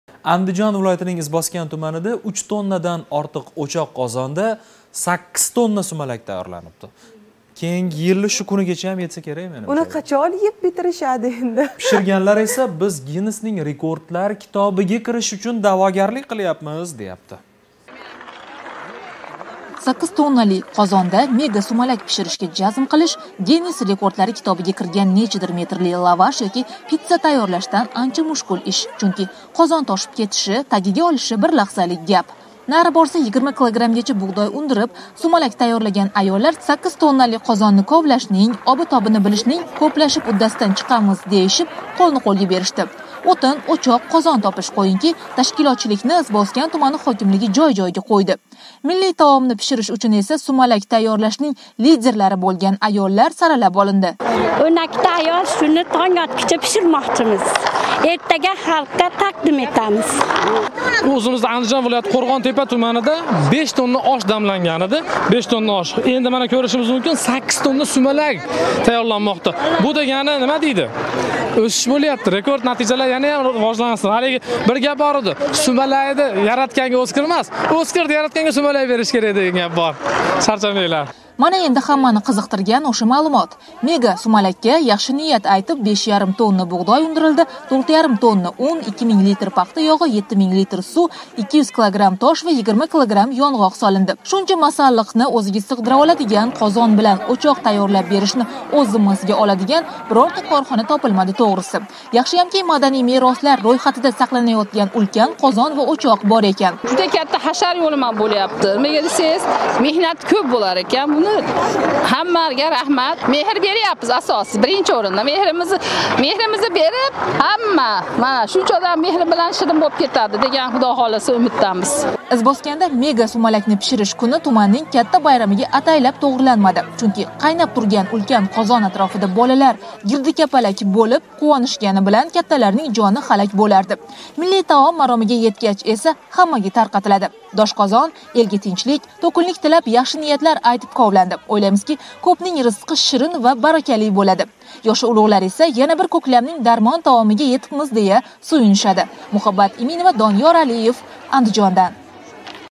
ИЗБОСКАНДА 8 ТОННАЛИК МЕГА СУМАЛАК ТАЙЁРЛАНДИ 3-лавҳа
⚡️Байрам байрамдек бўлсин дея, "Markaziy studiya" ҳам Избосканда тайёрланган 8 тонналик мега сумалак ҳақида лавҳа тайёрлаб, эфирга узатди.